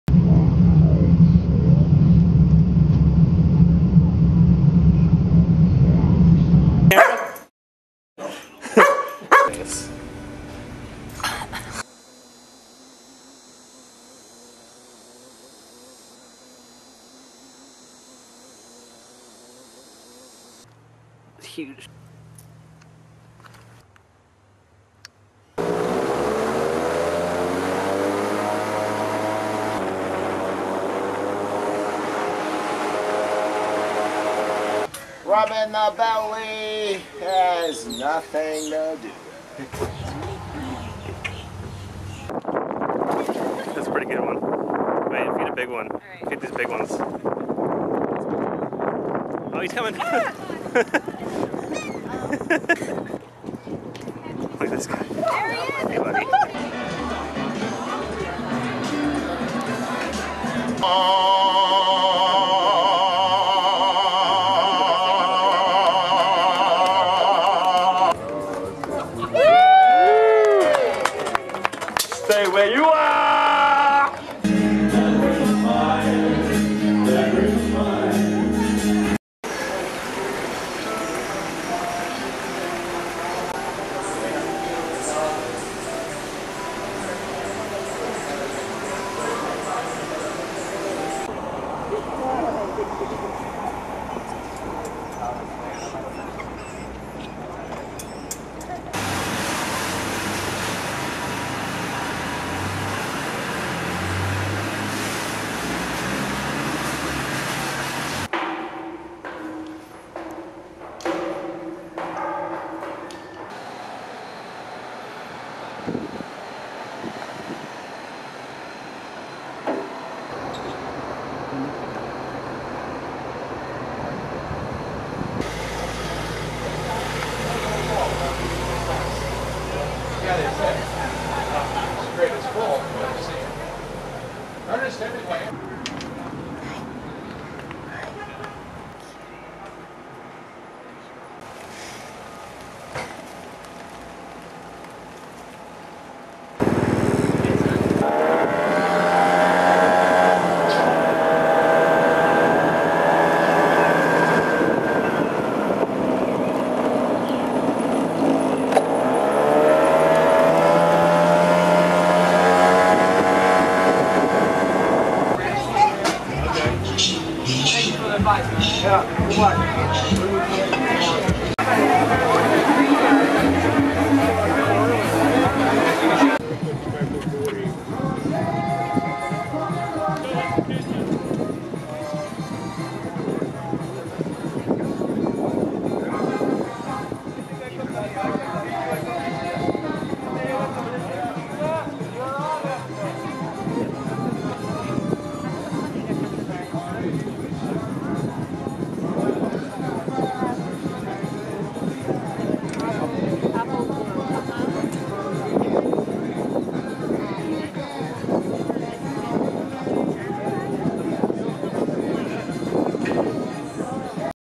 Florida trip
A short compilation of interesting moments from our vacation at the keys 10/11-10/19.  Filmed on my new camera, Lumix DMC-GX7.  I did a lot of software shake removal which is unfortunately noticeable in a few clips but you gotta work with what you got.  I also took the opportunity to learn how to do that sweet motion tracking text that was popular in videos a couple of years ago.